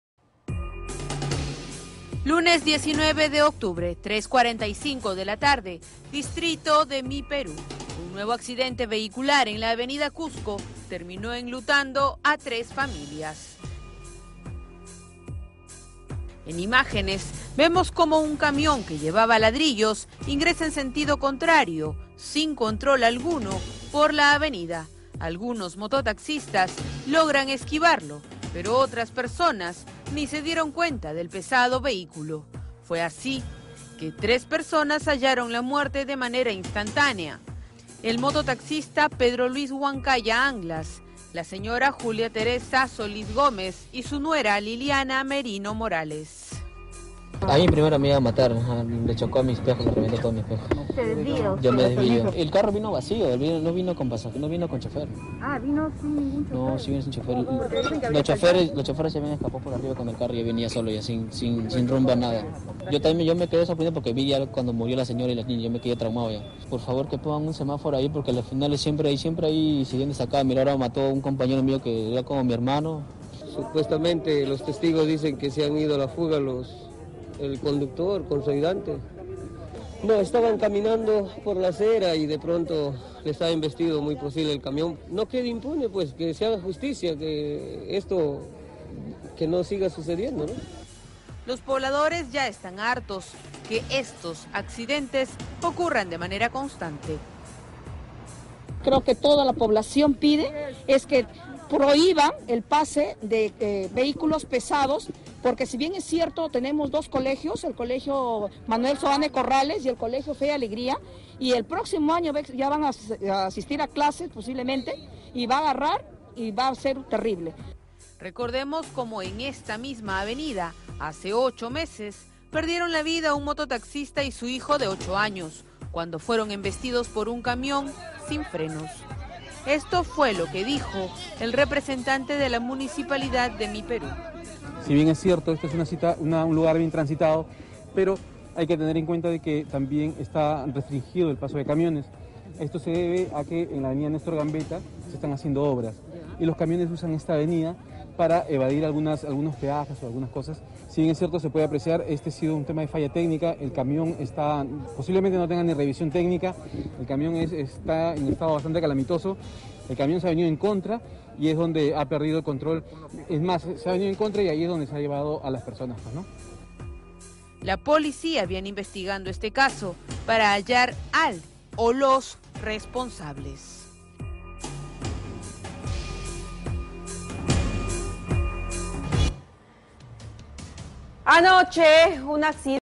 Camión fuera de control mata a 3 personas en Mi Perú (1/2)
Un nuevo accidente vehicular en la avenida Cusco, en el distrito de Mi Perú, en el Callao, cobró la vida de al menos tres personas. Los fallecidos fueron identificados como Pedro Luis Huancaya Anglas (conductor de mototaxi), Julia Teresa Solis Gomez y su nuera Liliana Merino Morales.

Imágenes captadas por las cámaras de vigilancia muestra cómo un camión fuera de control ingresa en sentido contrario por la avenida mientras algunos mototaxistas logran esquivarlo.

Como se recuerda en esta misma avenida, hace 8 meses, perdieron la vida un mototaxista y su hijo de 8 años, luego de ser embestidos por un camión sin frenos. La policía viene investigando este caso para hallar al responsable del accidente.